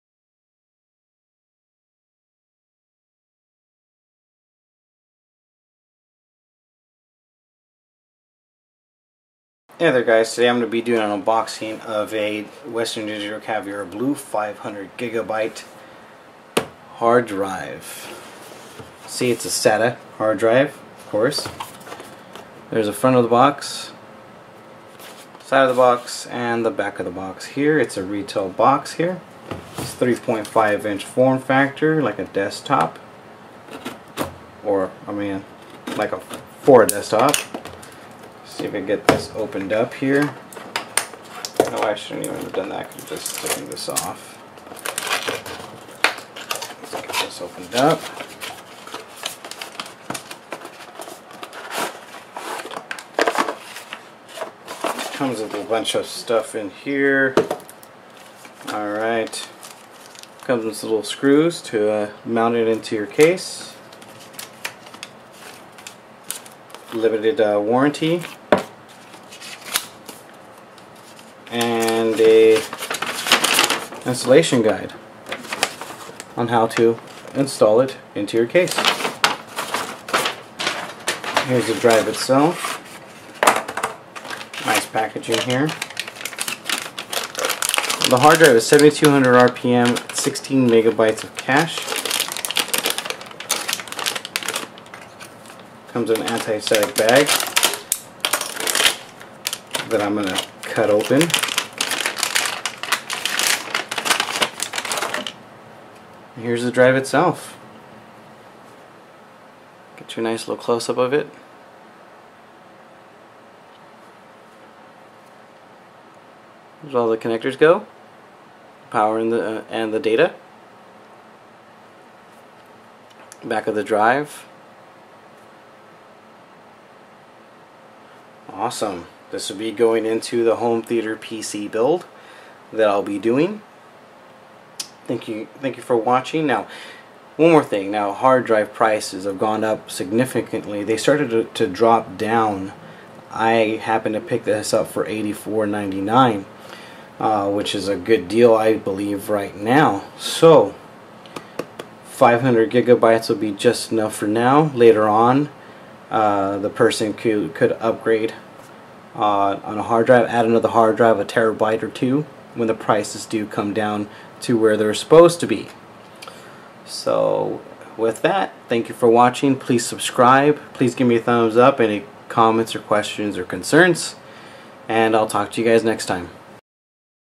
Unboxing WD Caviar Blue 500GB HDD
I unbox a WD Caviar Blue 500GB HDD for a HTPC!

Model

Brand
    Western Digital

Series
    Caviar Blue

Model
    WD5000AAKX

Packaging
    Bare Drive

Performance

Interface
    SATA 6.0Gb/s

Capacity
    500GB

RPM
    7200 RPM

Cache
    16MB

Physical Spec

Form Factor
    3.5"

Features

Features
    Solid performance - WD Caviar Blue drives are built to deliver rock-solid performance ideal for family and business computing.

    Ultra cool and quiet - Keeping the drive cool enhances reliability. WD continues to develop new and innovative ways to keep drives cool while they are operating which helps protect the drive and the data stored on it.

    NoTouch ramp load technology - The recording head never touches the disk media ensuring significantly less wear to the recording head and media as well as better drive protection in transit.

    IntelliSeek - Calculates optimum seek speeds to lower power consumption, noise and vibration.

    ShockGuard - Instantaneously protects the hard drive against damage from bumps and vibrations while it is running. This technology enables WD Caviar Blue drives to achieve industry-leading shock specifications.

    Compatible - WD perform tests on hundreds of systems and a multitude of platforms in our FIT Lab and Mobile Compatibility Lab to give our customers confidence that our drives will work in their systems.

Manufacturer Warranty

Parts
    2 years limited

Labor
    2 years limited